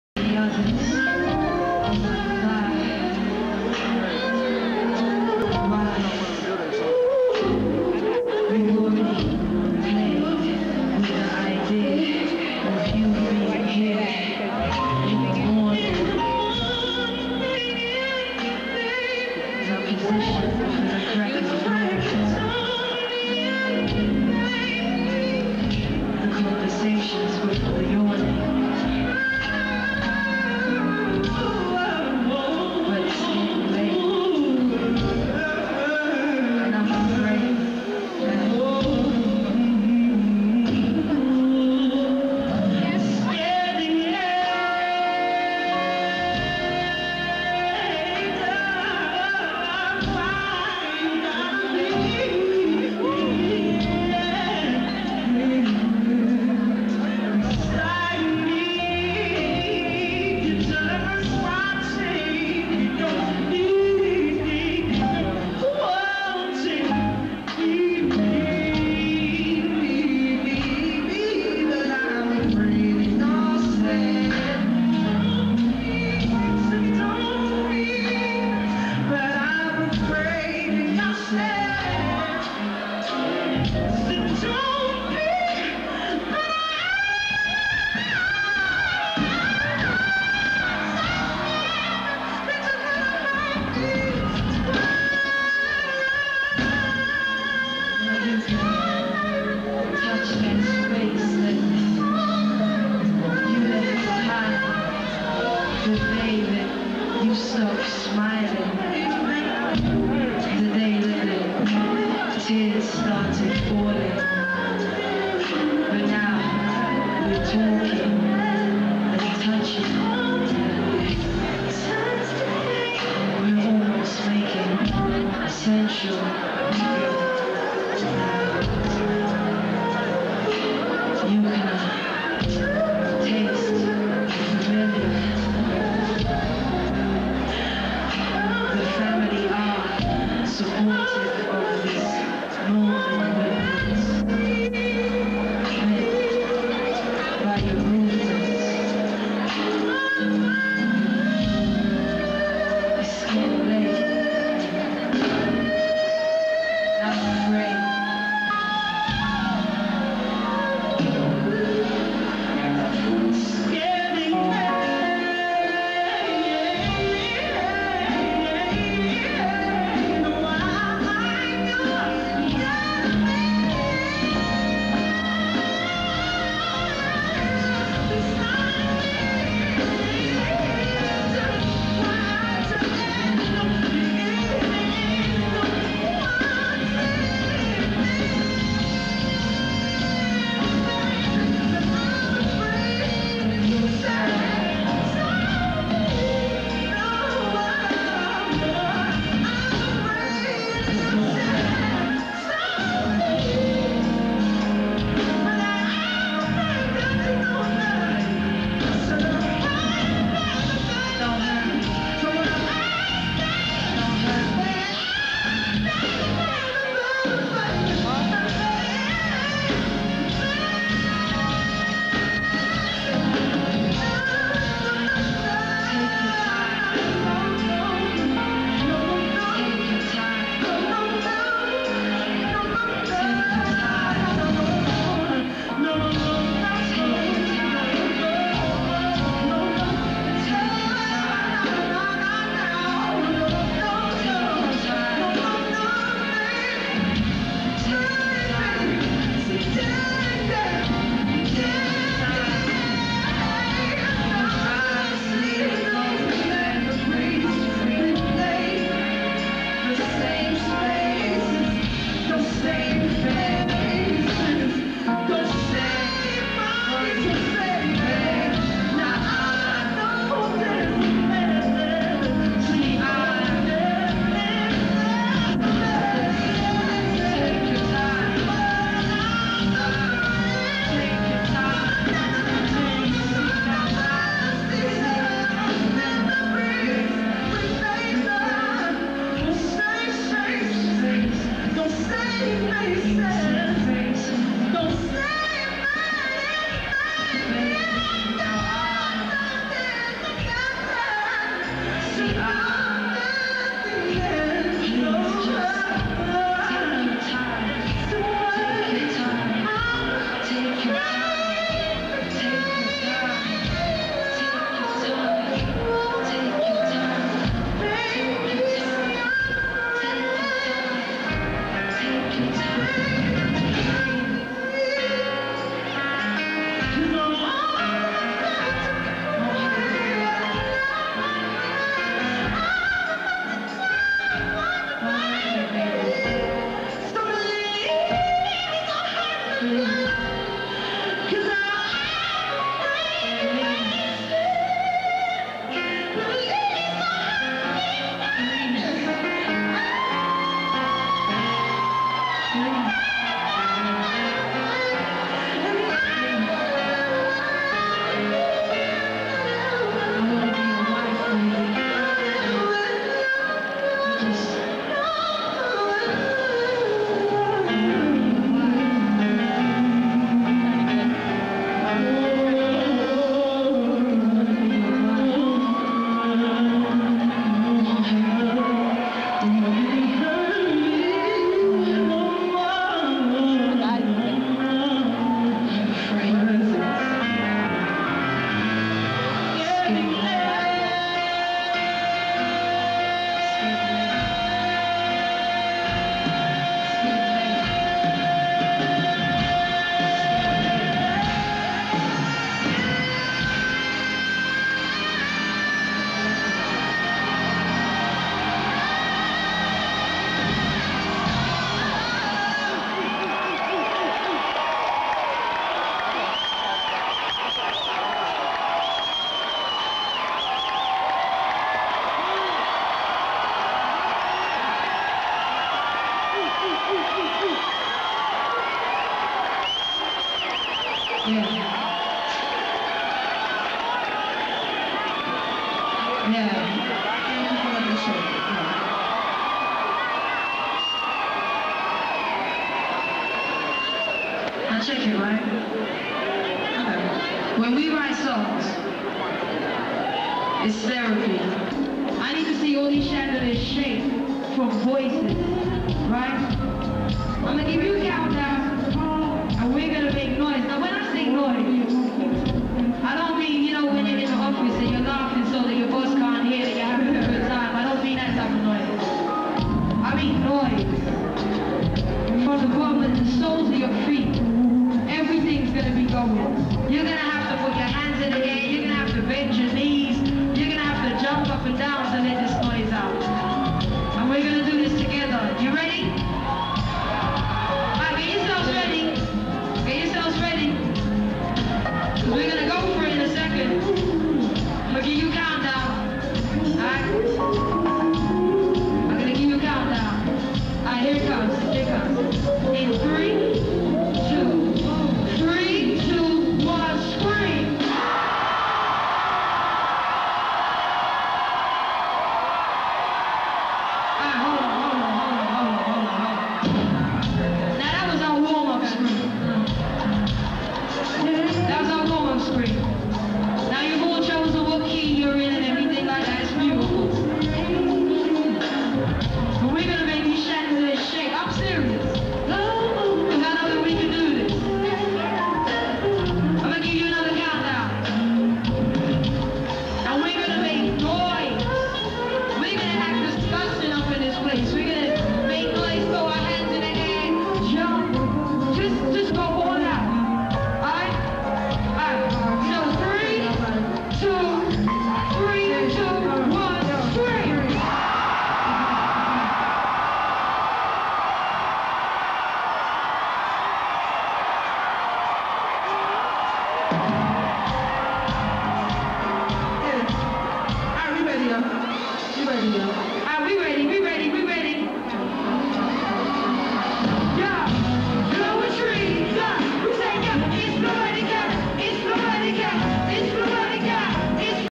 Floetry w/George "Spanky" McCurdy, Jeff Bradshaw, Thaddaeus Tribbett , Parris "Diddy" Bowens Live In Denver Colorado @ the Filmore Pt 4
Floetry Live In Denver,Co @ the Filmore. The band is made up of Tye Tribbet's band sound check & Jeff Bradshaw on Trumbone